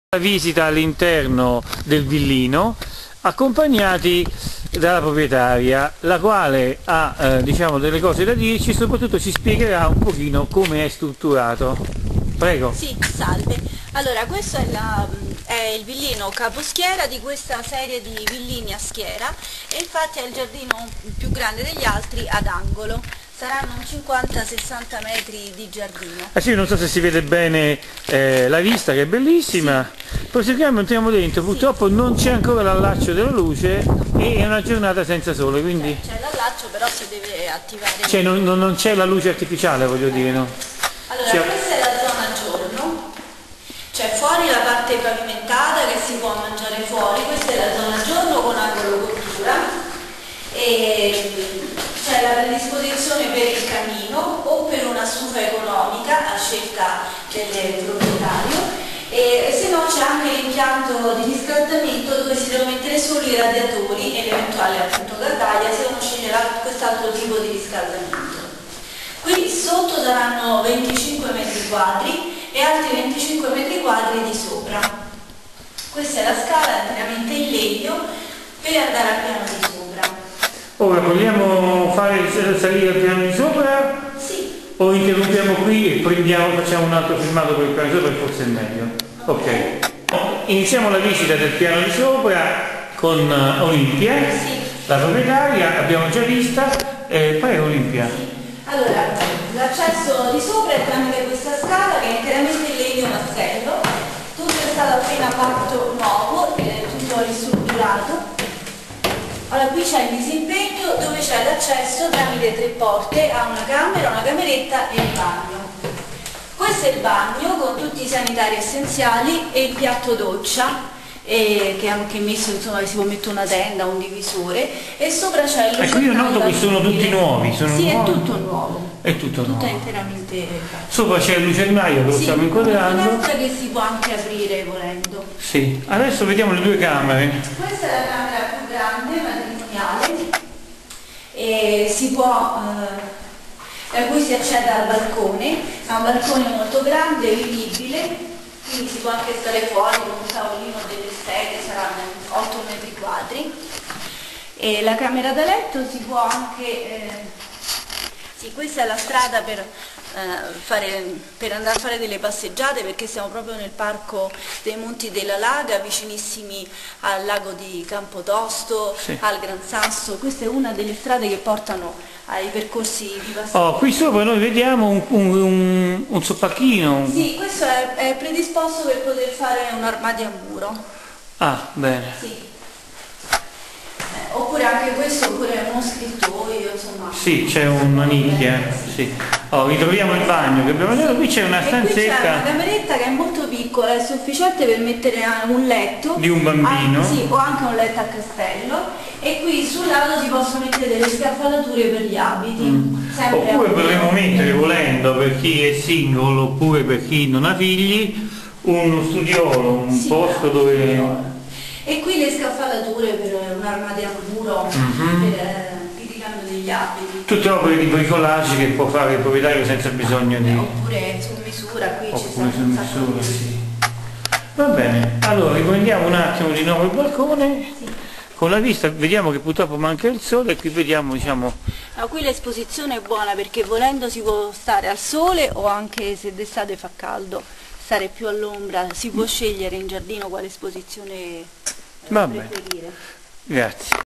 Amatrice (RI), Rifugio nei monti con tutti i comfort
Amatrice è una località nota a tutti, non solo per il sugo, ma per i monti della Laga, il lago di Campotosto, e per l'altitudine di circa 1.000 msl, che offre d'estate un fresco senza rivali nel Lazio, ed uno splendido paesaggio innevato già da Natale. Il prezzo la fa concorrere con una roulotte, sembra assurdo vero?